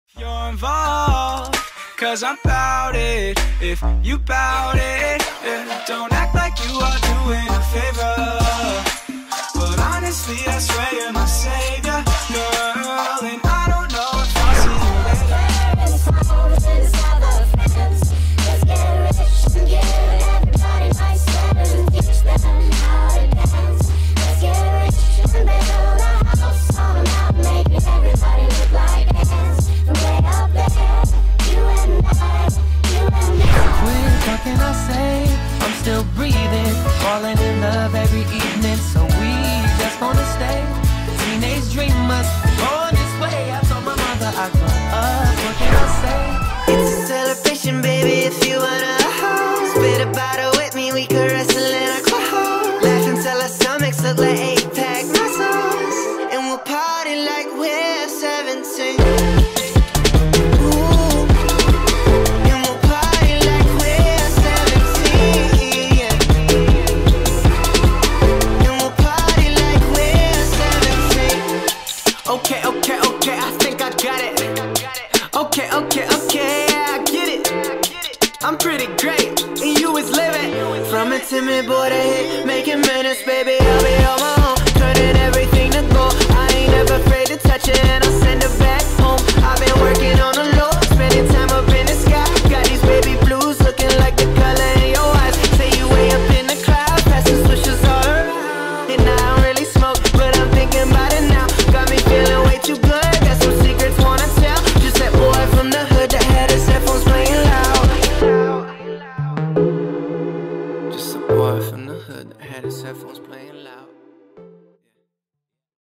Hanna Orthmann best aces so far in hip hop style
I migliori servizi vincenti della schiacciatrice della Saugella Monza messi a segno finora nella Serie A1 femminile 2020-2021. Dopo 12 giornate Orthmann è la migliore battitrice di Monza, terza del campionato in questo fondamentale, con 17 battute vincenti.